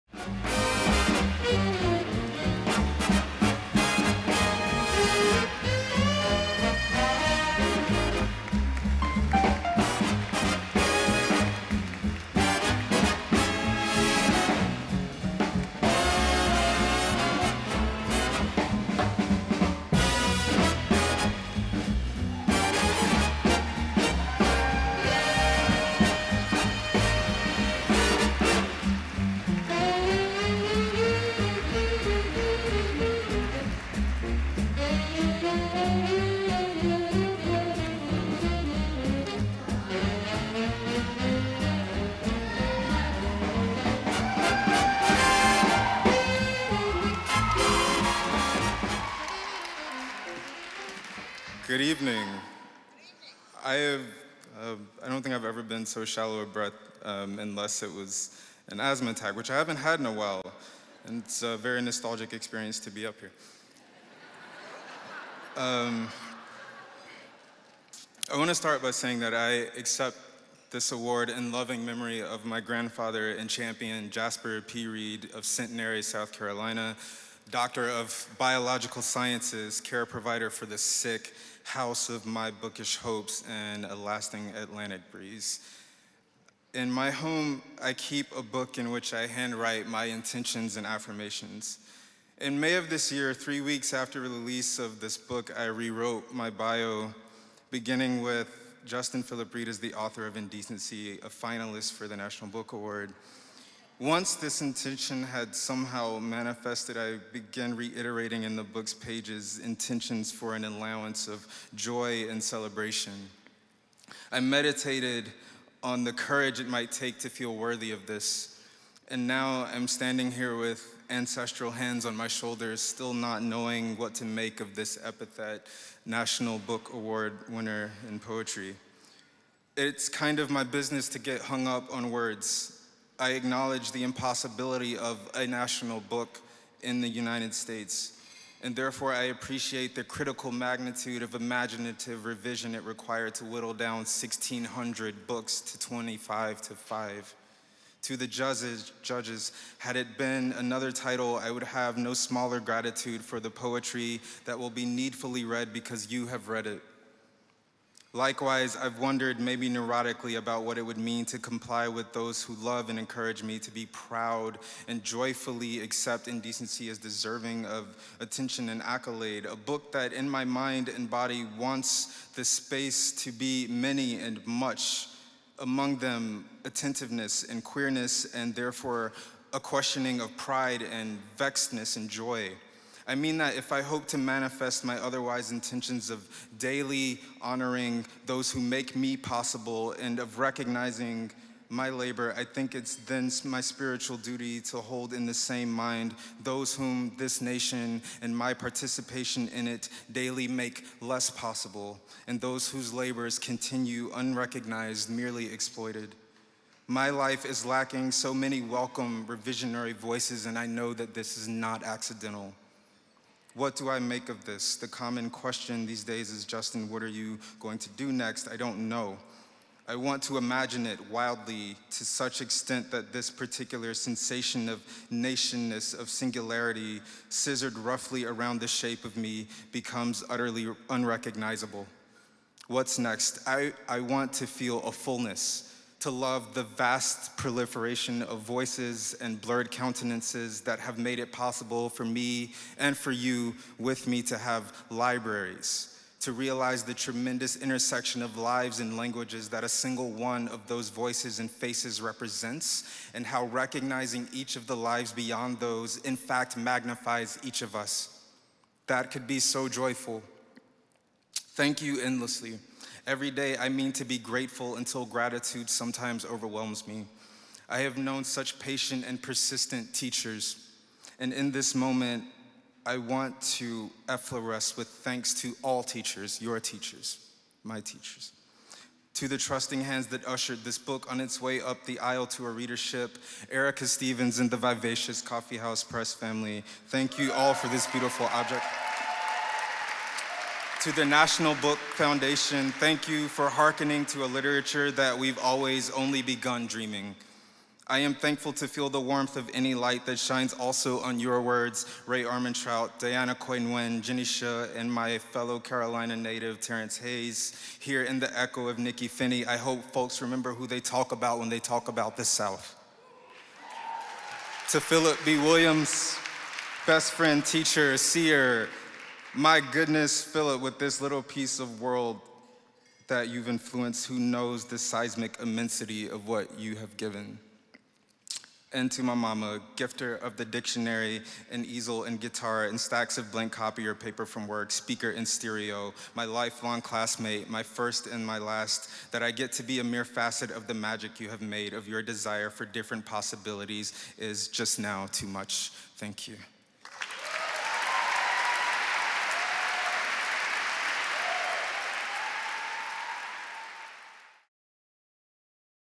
Author of Indecency Justin Phillip Reed accepts the 2018 National Book Award for Poetry
Justin Phillip Reed was presented the award by Mary Jo Bang, Chair of the 2018 NBA Poetry panel. More about Indecency by Justin Phillip Reed and the National Book Awards is available